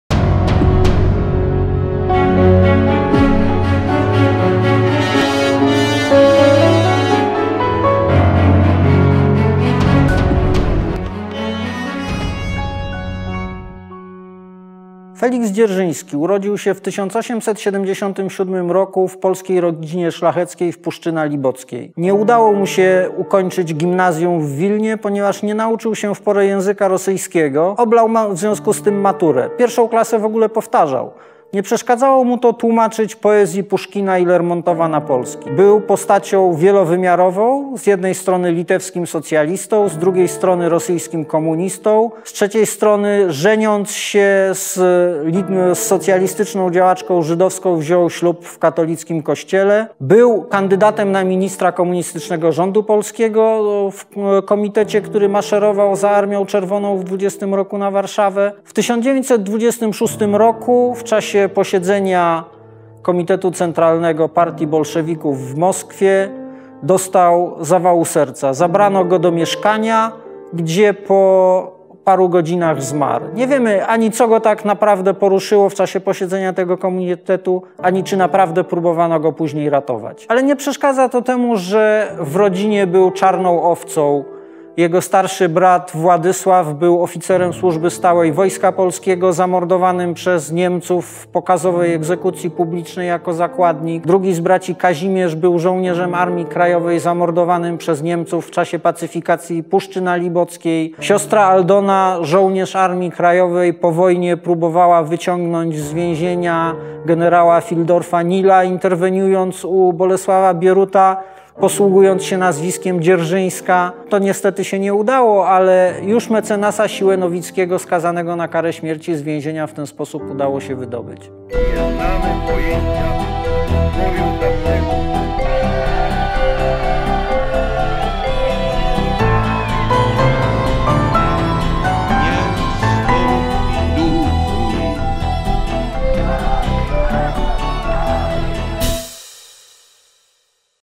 90s. historii: 20 lipca 1926 r. zmarł Feliks Dzierżyński
20 lipca 1926 roku zmarł Feliks Dzierżyński, komunista, socjaldemokrata, główny "architekt" sowieckiego terroru, szef pierwszych sowieckich organów bezpieczeństwa: Czeka, GPU i OGPU.